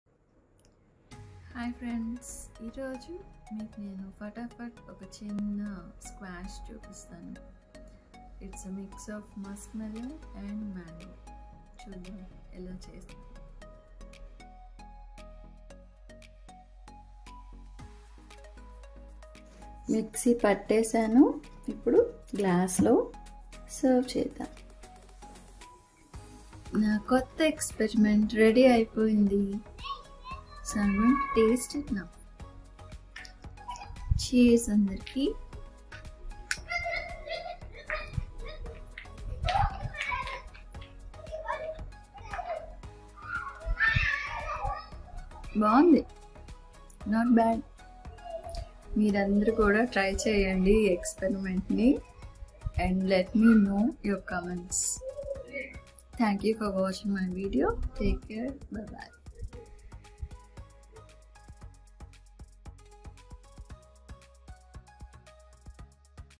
MANGO & MUSKMELON SQUASH | MANGO & MUSKMELON SMOOTHIE | MANGO JUICE| BEAT THE HEAT
This is the season for Mangoes. Mangoes are good for health rich in various vitamins, minerals and nutrients. Muskmelon helps the body from dehydration. There are many varieties of mangoes. Rasalu is the one which gives heaven like taste. Acquire the benefits of the fruits that grow in the particular seasons. Mangoes are not available in all the seasons. Mangoes are available only during the months of April and May in India. India is famous for producing the mangoes and the farmers export mangoes to other Countries also. Sow a mango seed in your garden, within 5 years it will give you good yield. Eat good healthy food and stay fit. Instead of junk food children can be given a mango fruit to have during the snack time. Enjoy mangoes in the summer.

| 3 STEPS TO COOK THIS YUMMY JUICY PRAWNS CURRY | MOUTH WATERING SHRIMP CURRY | DIY AT HOME PARTY